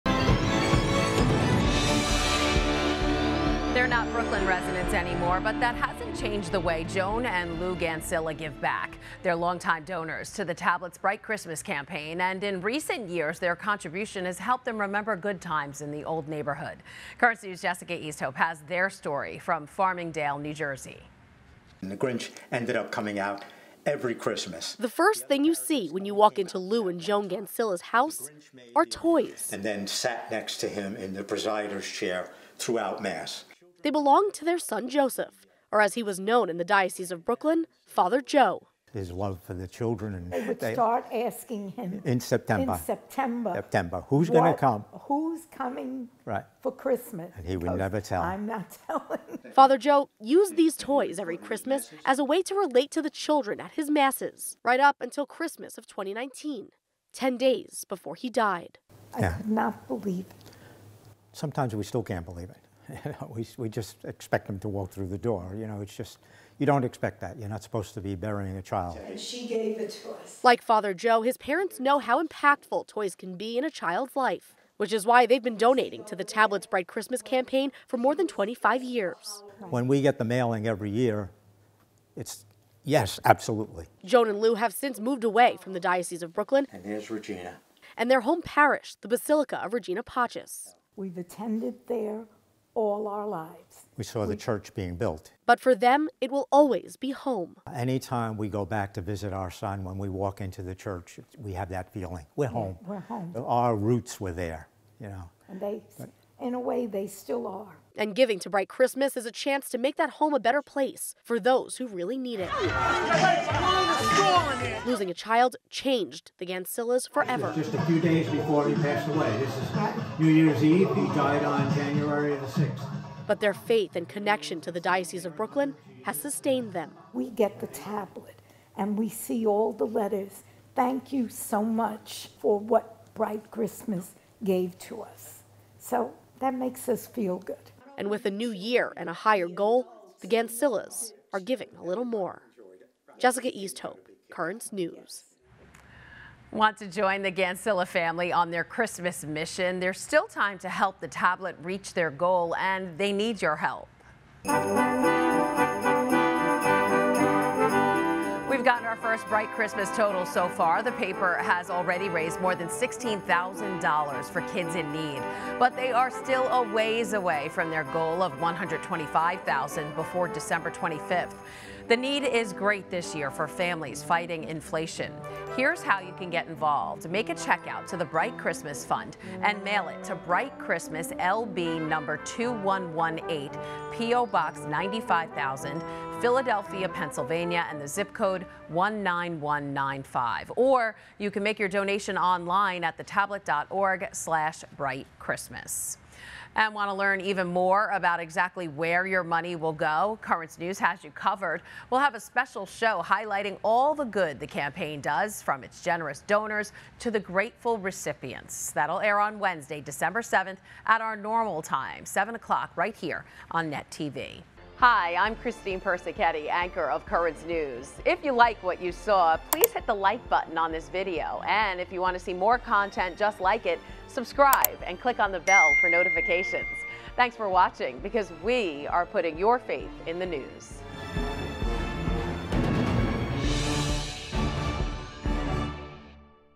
Parents of Late Diocese of Brooklyn Priest Donate to Bright Christmas Campaign
They're not Brooklyn residents anymore but that hasn't changed the way Joan and Lou Gancila give back. They're longtime donors to the Tablet's Bright Christmas campaign and in recent years their contribution has helped them remember their late son, a Diocese of Brooklyn priest who used toys as a way to children’s hearts during Christmas.